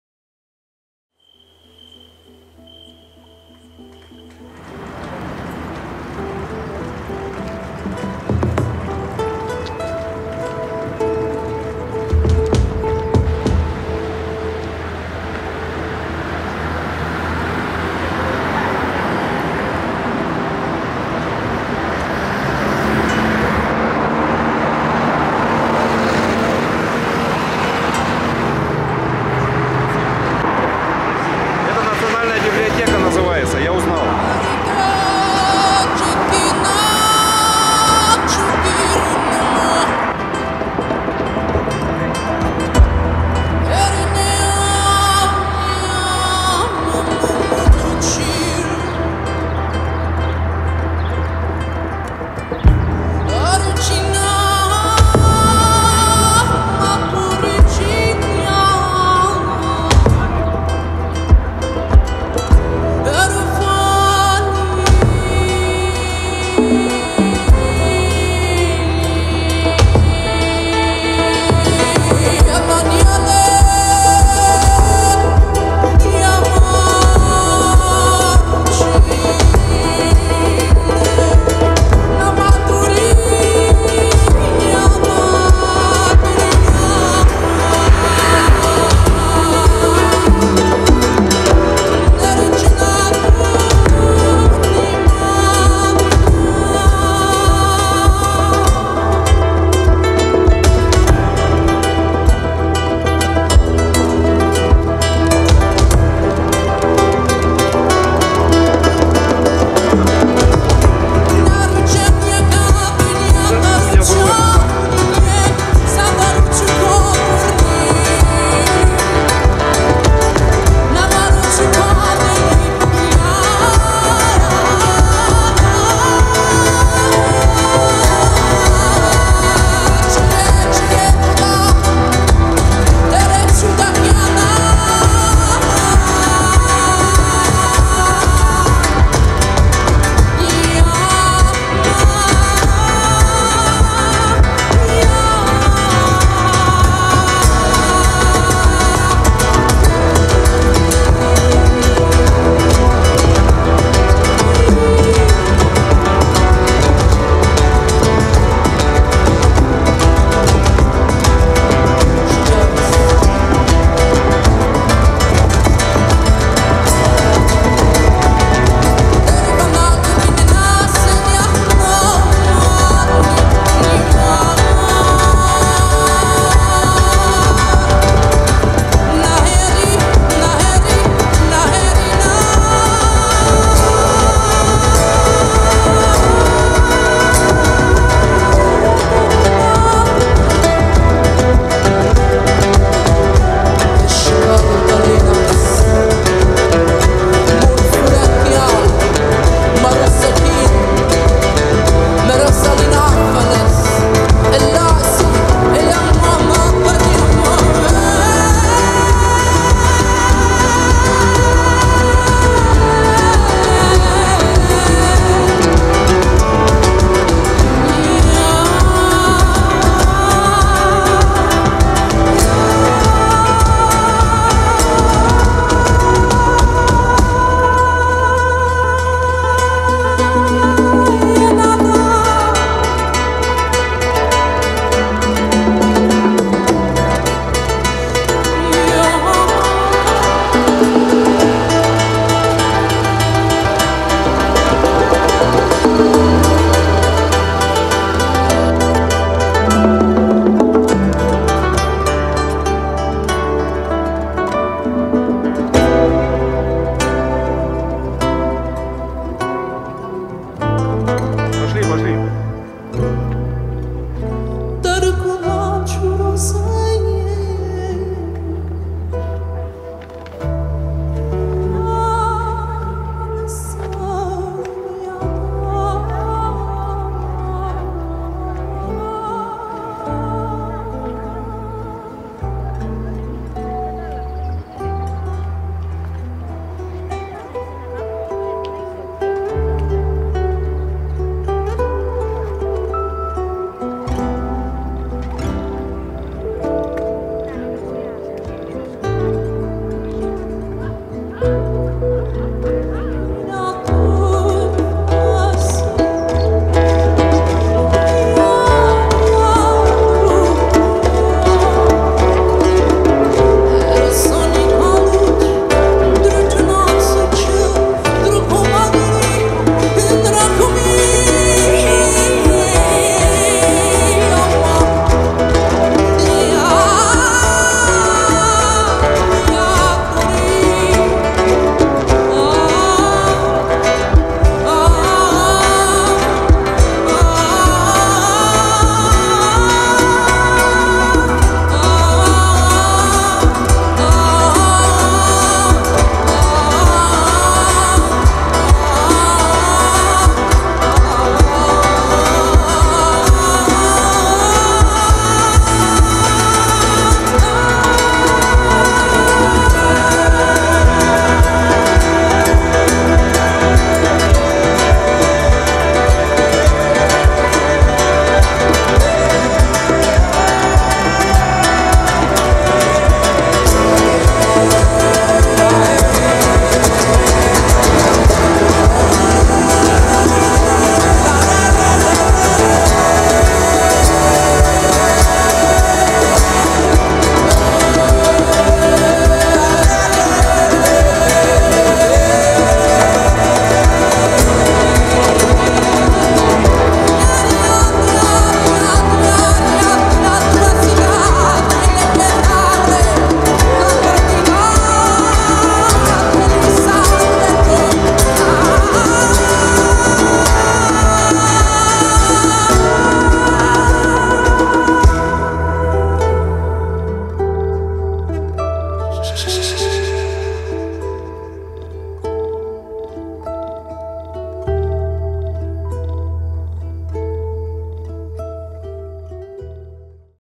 Греция 28 апреля - 14 мая 2018 Вечерние Афины, Mount Lycabettus
33-я Русско-Эгейская регата
Снято Sony FDR AX53